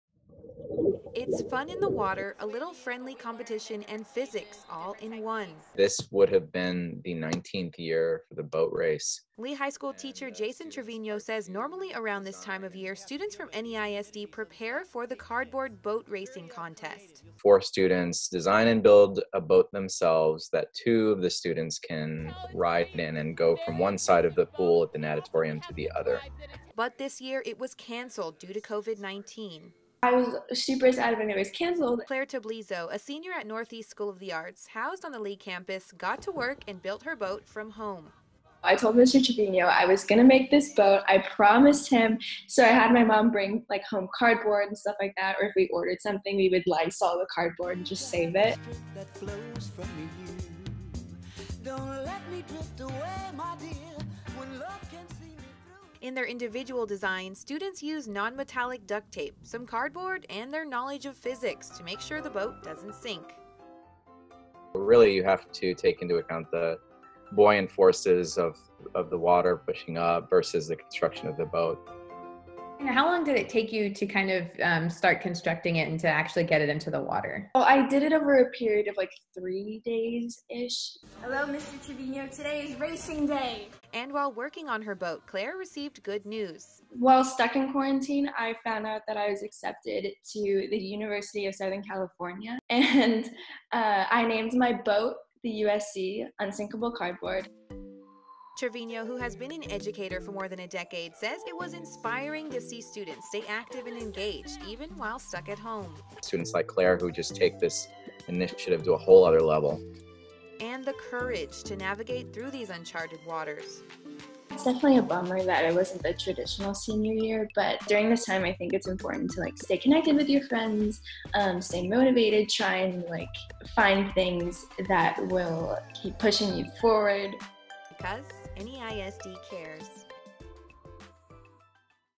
Physics student continues District tradition from home
It’s fun in the water, a little friendly competition, and physics all in one.

“This would have been the 19th year of the boat race,” LEE High School Physics Teacher Jason Trevino said.

Normally around this time of year, students from around NEISD prepare for the cardboard boat racing contest.

“Four students design and build a boat themselves that two of the students can ride in and go from one side of the pool at the Natatorium to the other. They race along with other students in the District that are all physics students,” Trevino explained.

Unfortunately, this year it was canceled due to COVID-19.

“I was super sad when it was canceled,” Claire Tablizo, a senior at North East School of the Arts (NESA), which is housed on the L.E.E. campus, said.

“I told Mr. Trevino I was going to make this boat, I promised him,” she said.

So, Claire got to work and built her boat from home.

“I had my mom bring home cardboard. Or, if we ordered something, we would Lysol the cardboard and just save it,” Tablizo added.

In their individual designs, students use non-metallic duct tape, some cardboard, and their knowledge of physics to make sure the boat doesn’t sink.

“Really you have to take into account the buoyant forces of the water pushing up versus the construction of the boat,” Trevino explained.

Tablizo said it took about three days to complete her canoe-style boat.

“It took way longer than I thought it would; there’s a reason why it’s a four-man job,” she said.

While working on her boat, Claire also received good news.

“While stuck in quarantine, I found out that I was accepted to the University of Southern California for musical theater. I named my boat USC: the UnSinkable Cardboard,” she said.

Trevino, who has been an educator for more than a decade, said it was inspiring to see students stay active and engaged even while stuck at home.

“With students like Claire who just take this initiative to a whole other level, it still gives me hope for everything to adapt regardless of how long this thing goes on,” he said.

It also gives them the courage to navigate through these uncharted waters.

“It’s definitely a bummer that it wasn’t the traditional senior year but I think you just have to focus on what’s happening in the future. Try to find things that will keep pushing you forward,” Tablizo said.

Because NEISD cares!